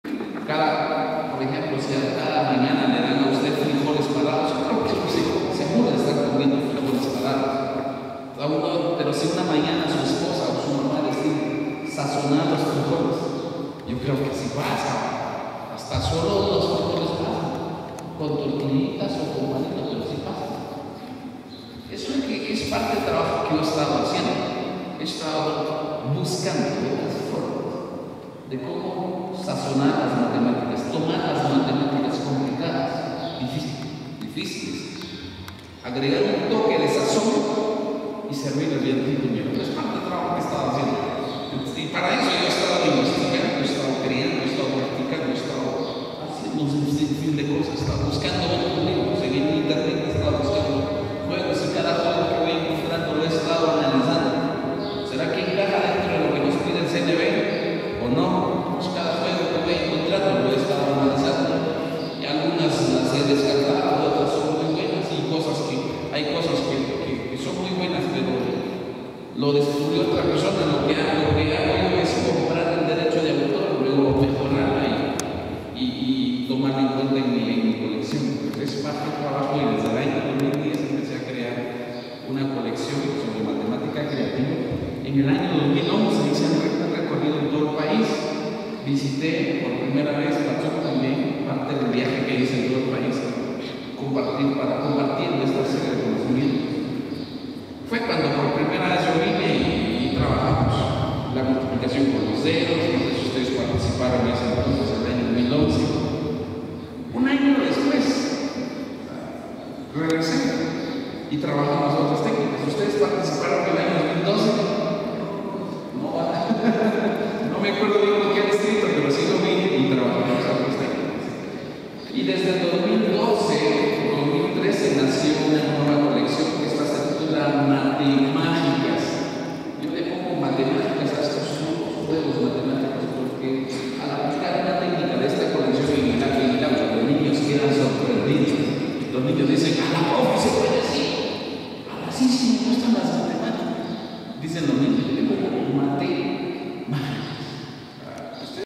Ambiente animado durante capacitación a maestros
Un animado ambiente compartieron maestros de varias escuelas de esta localidad durante la capacitación dirigida por el licenciado Darío Ordóñez, y llevado a cabo en el gimnasio municipal.